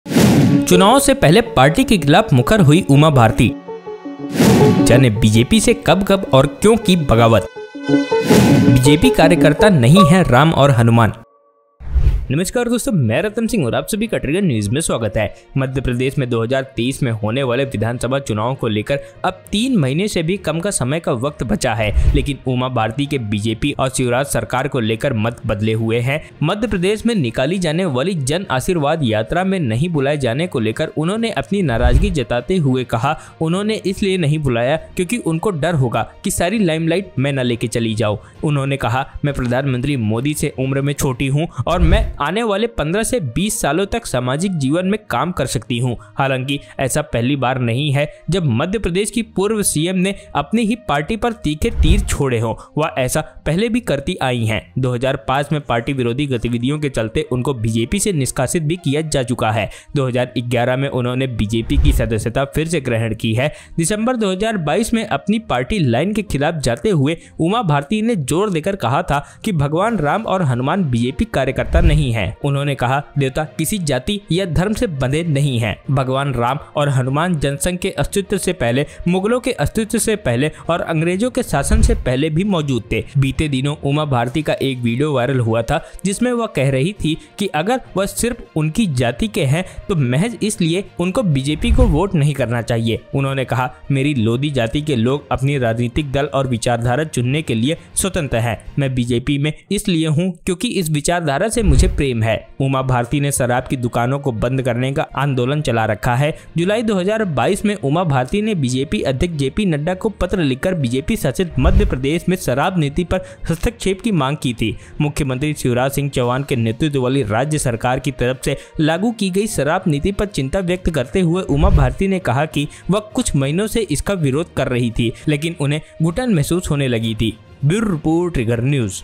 चुनाव से पहले पार्टी के खिलाफ क्यों मुखर हुईं उमा भारती | BJP कार्यकर्ता नहीं हैं राम और हनुमान
चुनाव से पहले पार्टी के खिलाफ क्यों मुखर हुईं उमा भारती | BJP कार्यकर्ता नहीं हैं राम और हनुमान | Trigger Newz